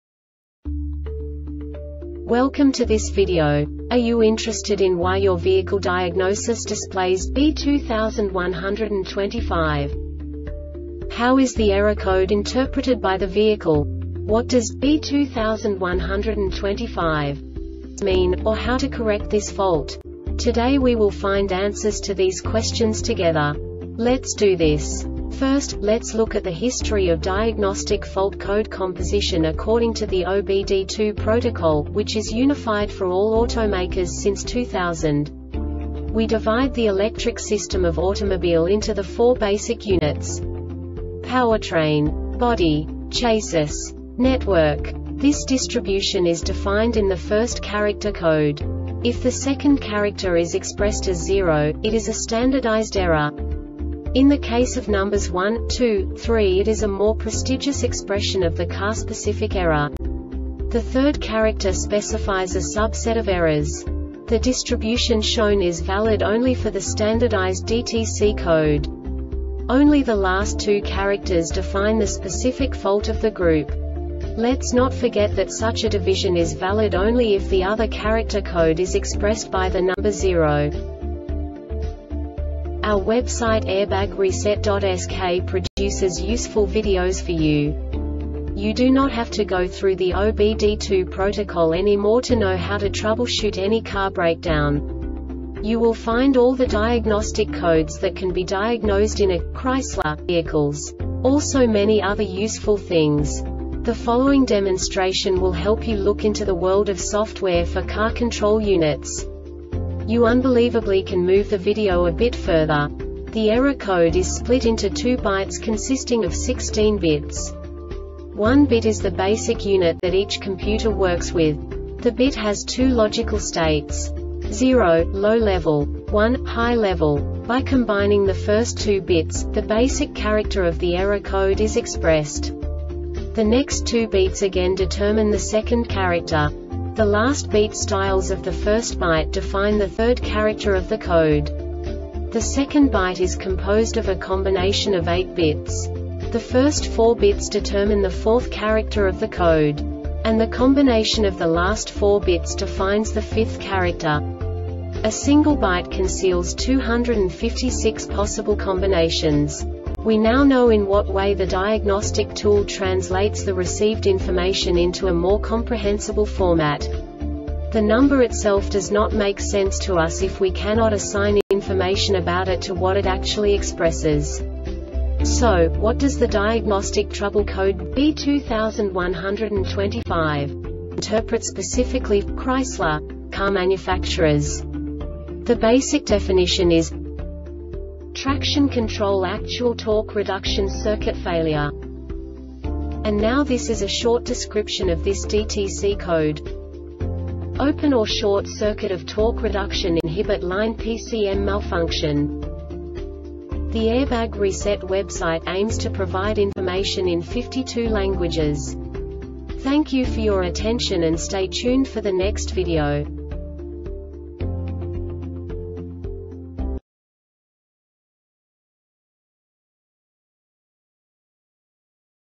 DTC Chrysler B2125 Short Explanation
Contents:
0:21 Basic DTC analysis according to OBD2 protocol standard.
1:48 Insight into programming
2:58 A diagnostically understandable description of the Diagnostic Trouble Code
3:05 Basic error definition

Make:
Chrysler
Code:
B2125
Definition:
Traction Control Actual Torque Reduction Circuit Failure
Cause:
Open or short circuit of torque reduction inhibit linePCM malfunction